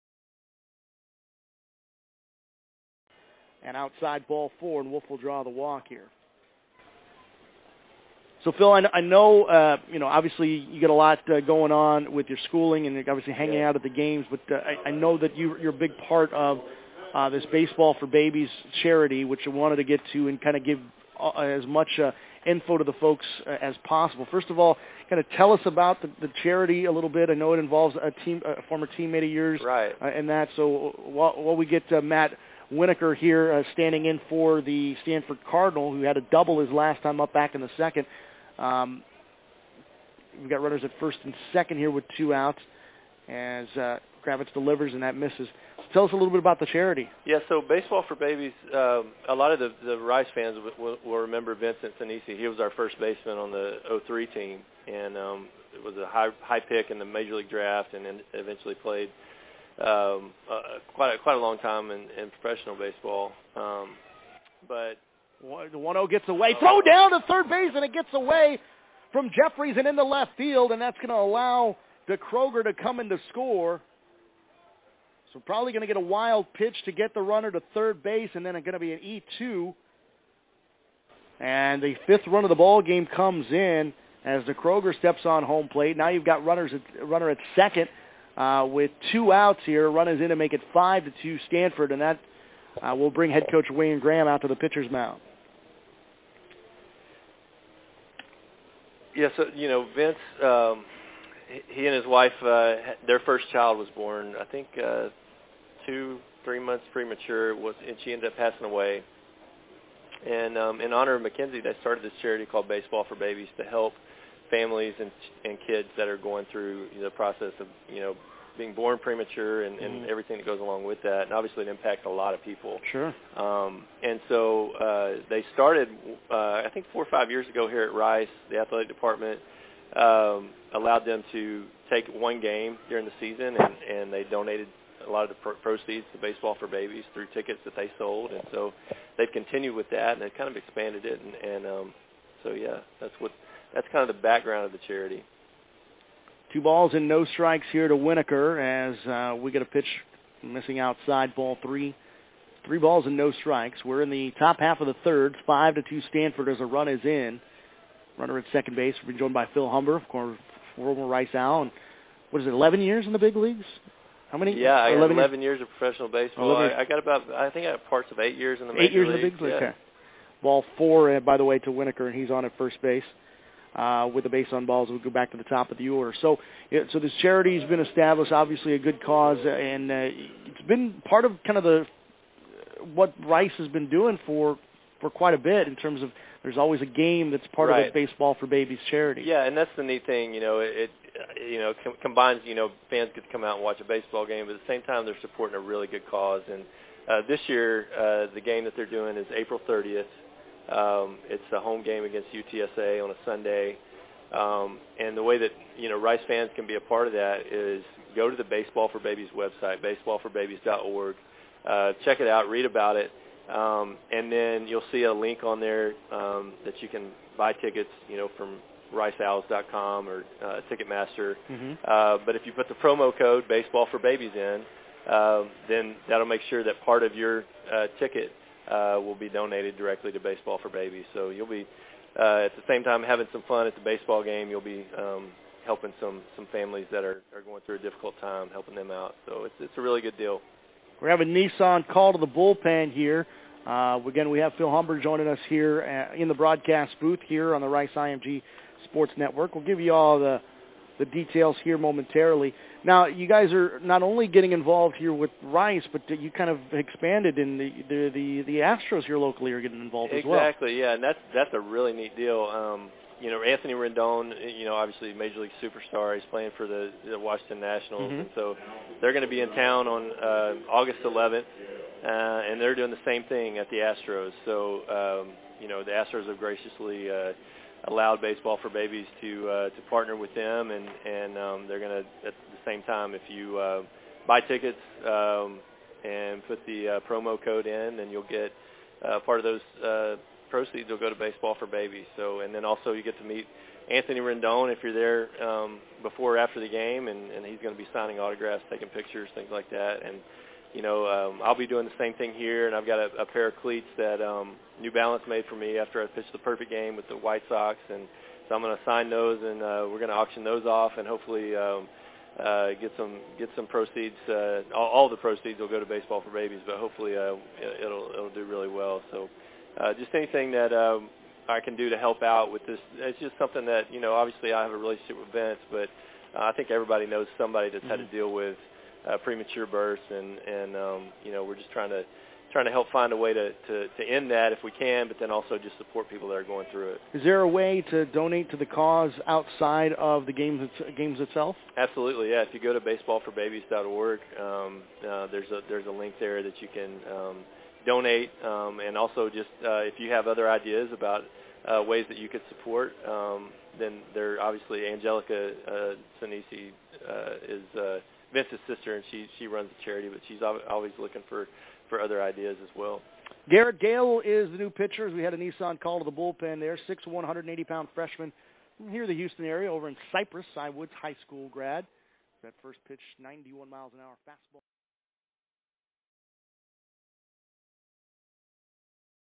A little baseball announcing and Baseball for Babies
I will be forever debted to Philip Humber!  This is so cool on all levels. A little Rice Owls baseball announcing and Baseball for Babies. Check it out!